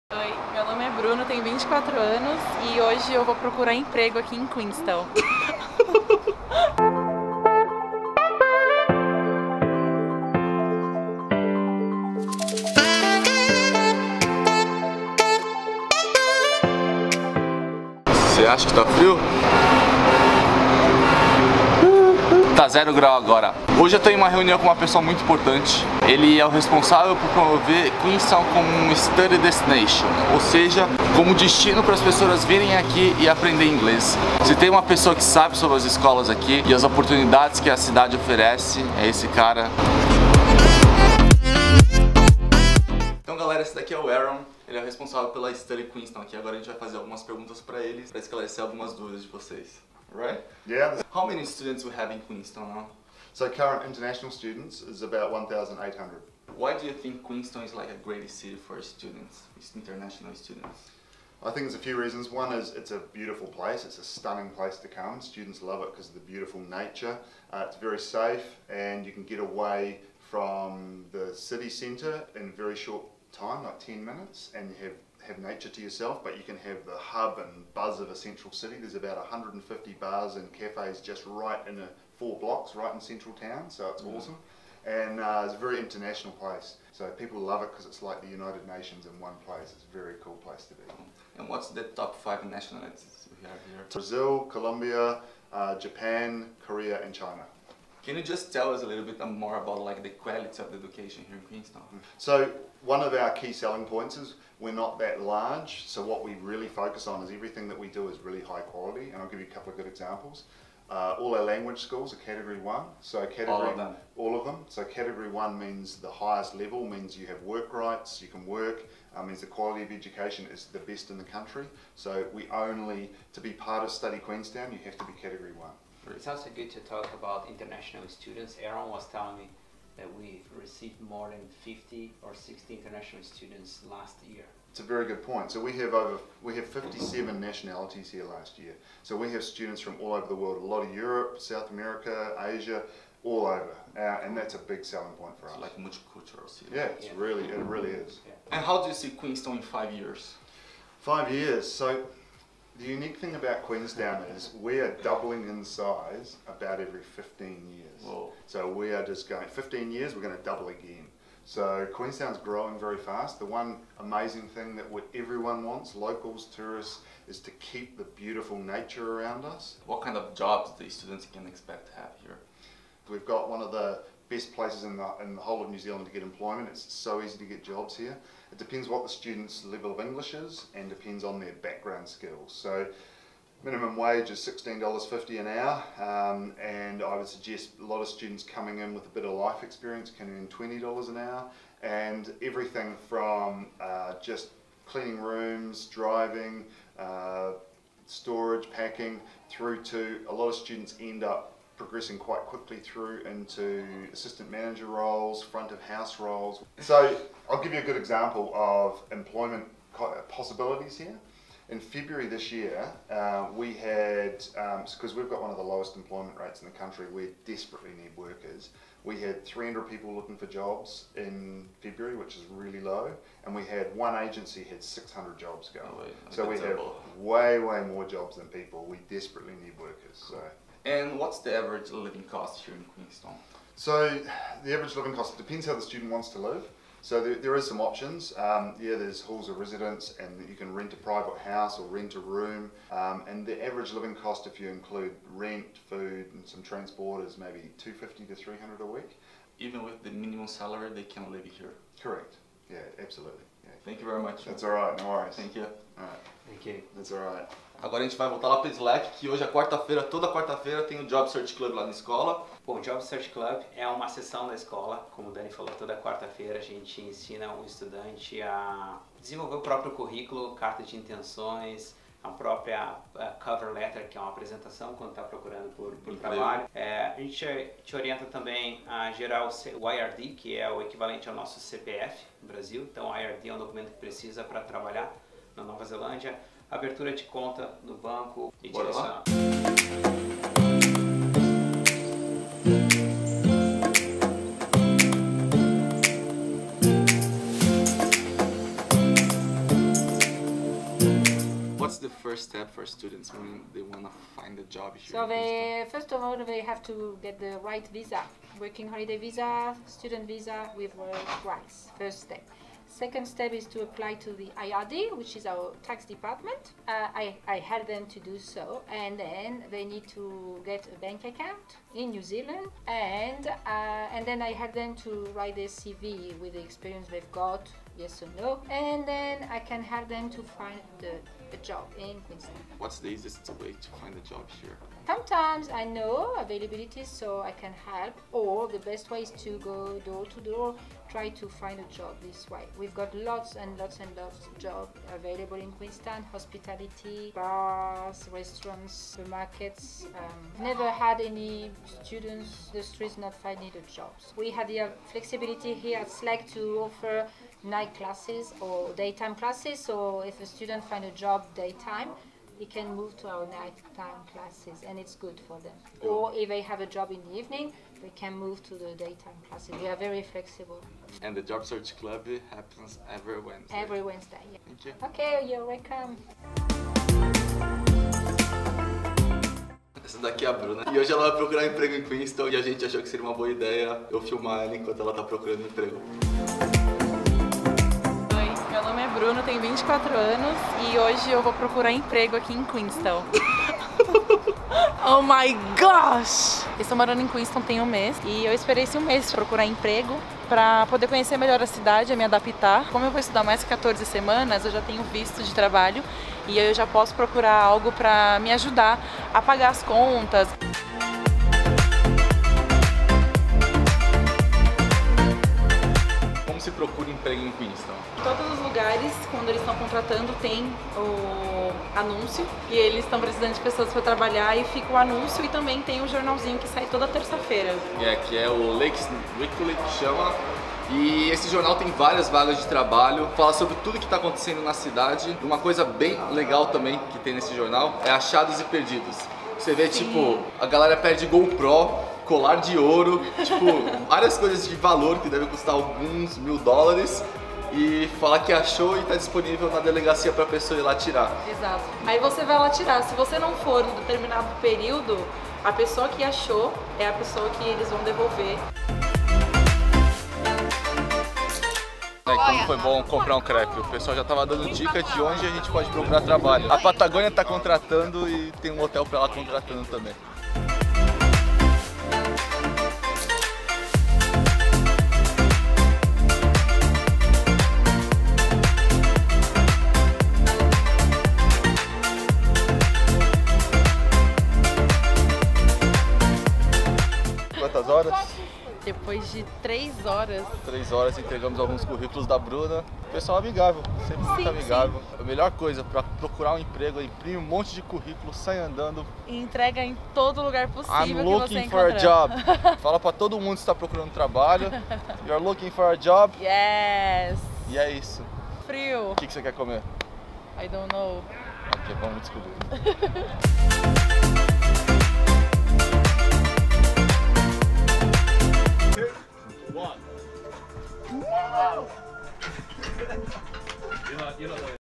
PROCURANDO EMPREGO NA NOVA ZELÂNDIA | EPISÓDIO 05 | DIÁRIO DE INTERCÂMBIO
Quer saber como é procurar emprego durante o seu intercâmbio em Queenstown na Nova Zelândia na escola Southern Lakes English College (SLEC)? Nesse dia também entrevistamos o responsável de promover a cidade como destino para as pessoas estudarem e acompanhamos uma estudante procurando um emprego!

Pensando em fazer um intercâmbio? Me manda uma mensagem que eu te ajudo com tudo!